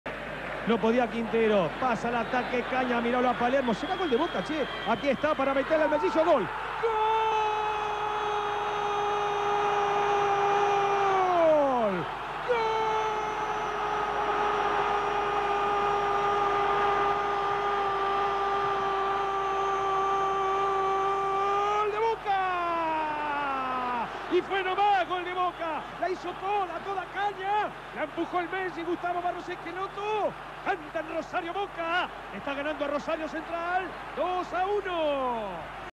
Gol de Gustavo Barros Schelotto a Central (Boca 3-Central 1 30-10-99)
SI TE GUSTÓ EL VIDEO, PODÉS COLABORAR CON UNA DONACIÓN

Gol de Gustavo Barros Schelotto a Central a los 61' por el Torneo Apertura 1999 (Boca 3-Central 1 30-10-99)